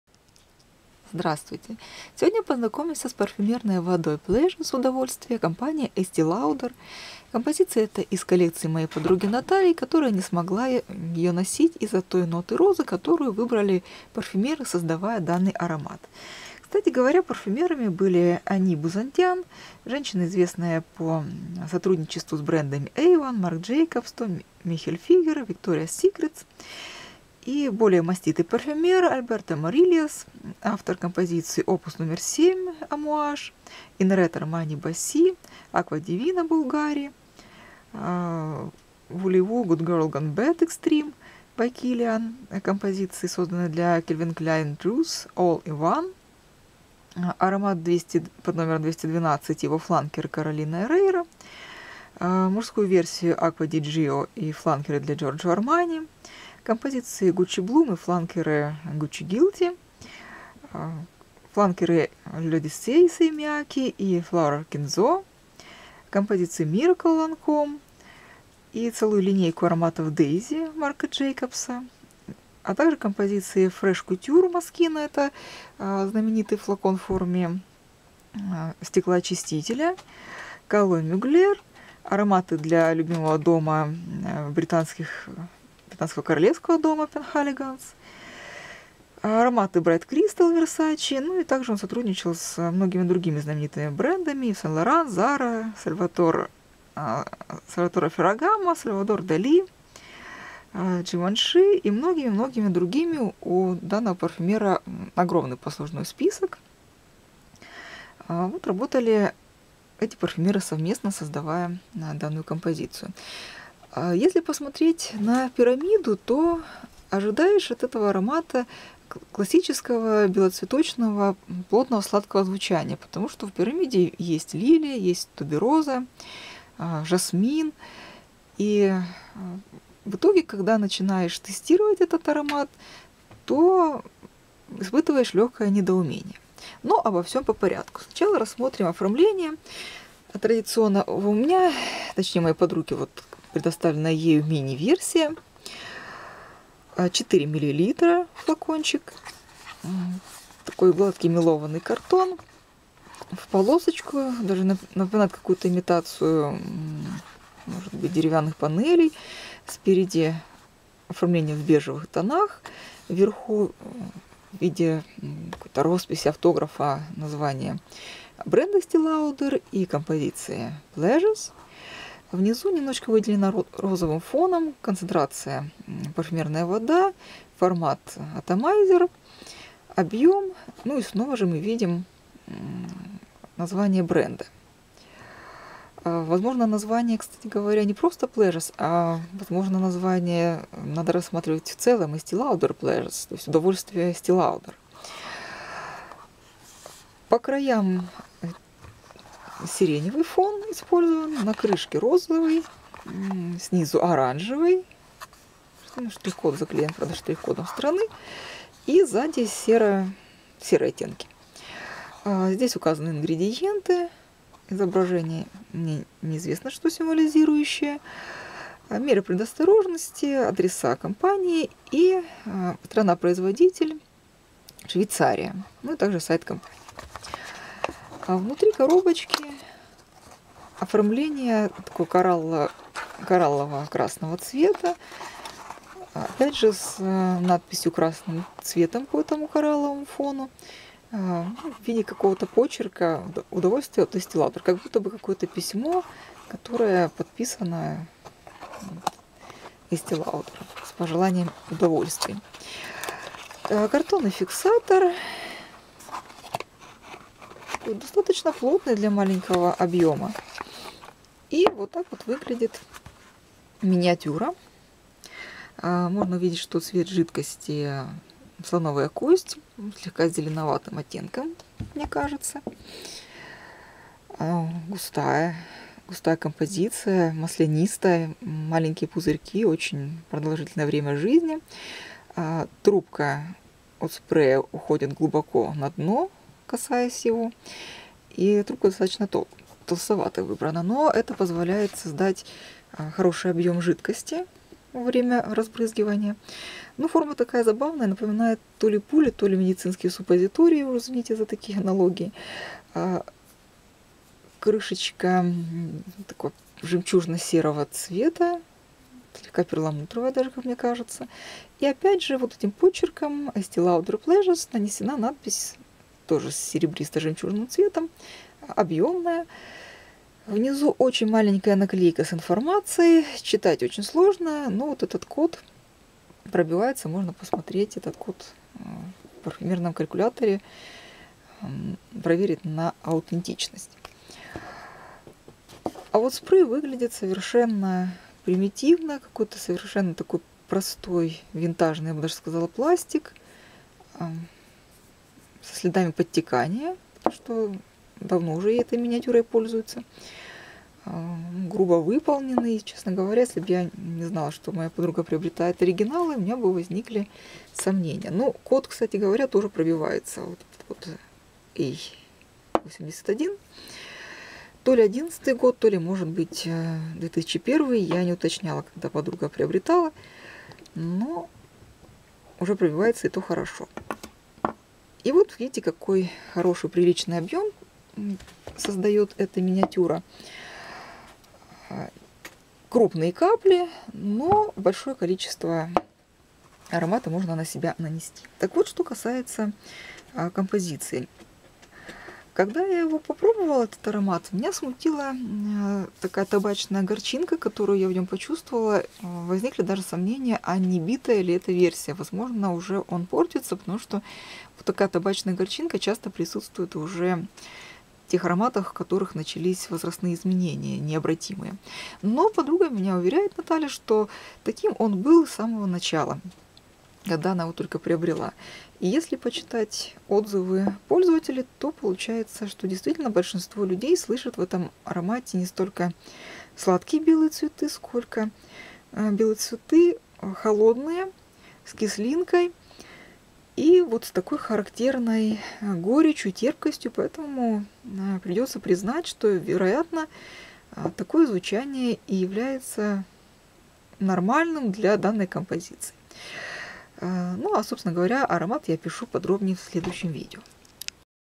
Estée Lauder Pleasures (Estée Lauder). Часть 1. Общие сведения, парфюмеры, дизайн
ПВ Estée Lauder Pleasures Estée Lauder
Общие сведения об аромате, парфюмерах. Дизайн упаковки и мини-флакона (4 мл)